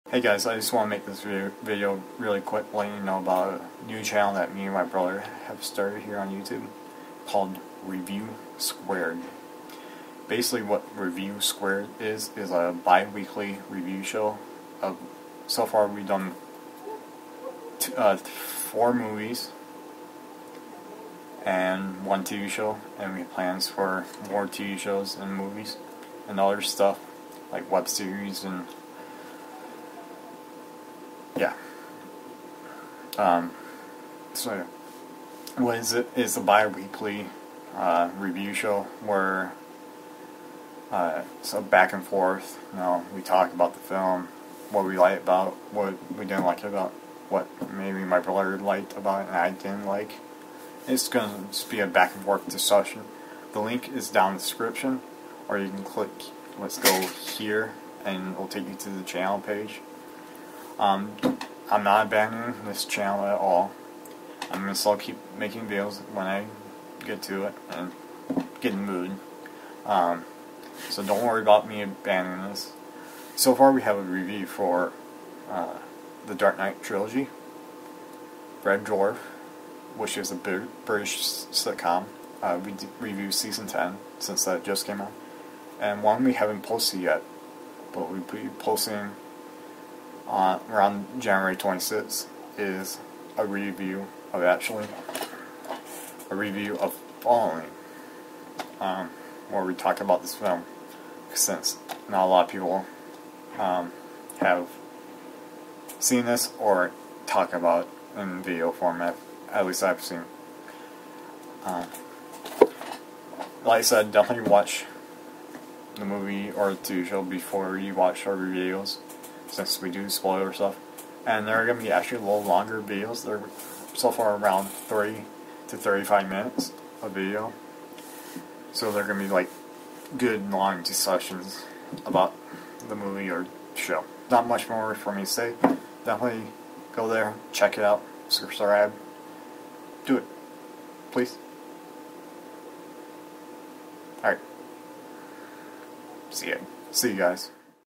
Reviewed Squared Introduction
This is a introduction to a channel that my brother and I started. I will keep making videos for this channel.